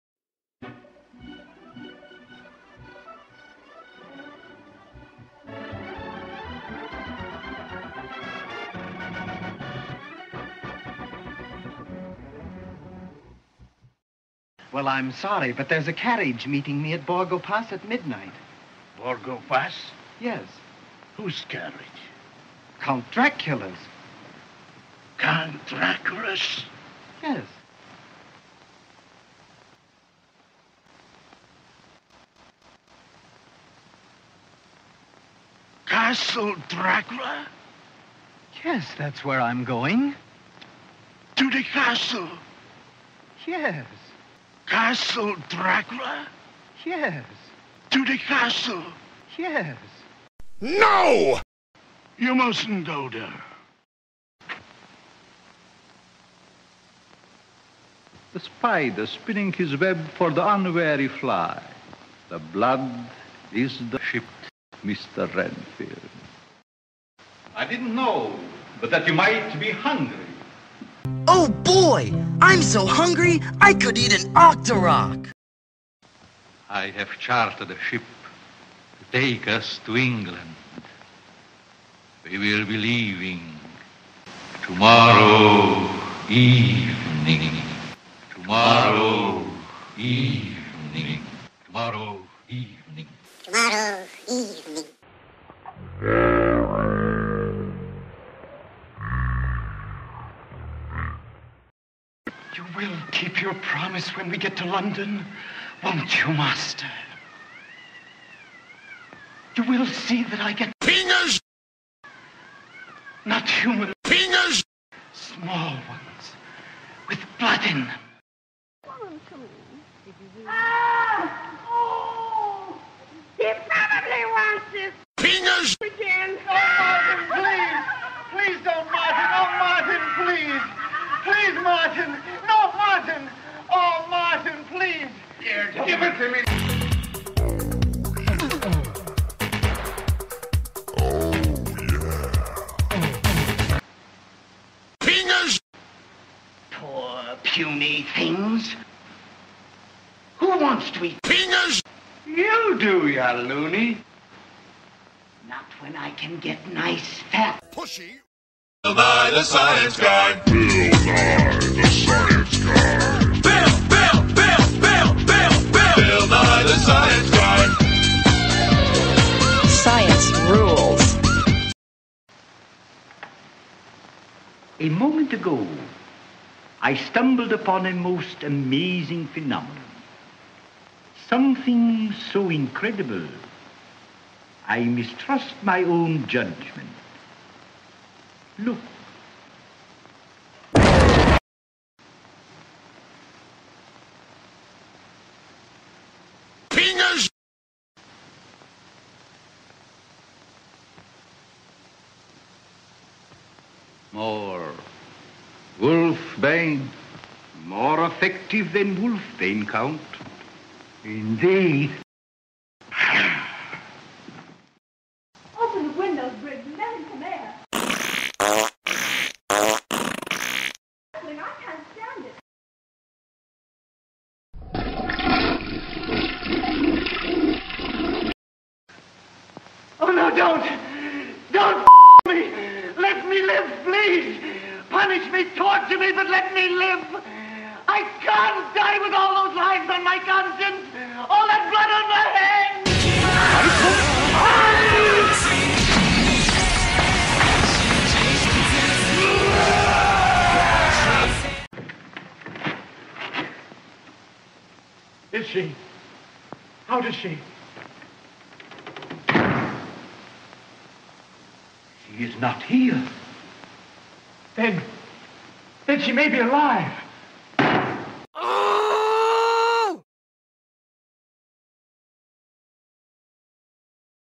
YouTube Poop - Dracula's Misadventure
What sort of "adventures" await Dracula in London? Renfield can't get enough PINGAS!

My first YTP! It took me about a day to make thanks to VideoPad and Windows Movie Maker 2.6. I had a great time making this video!

12/29/2010 : 100 views!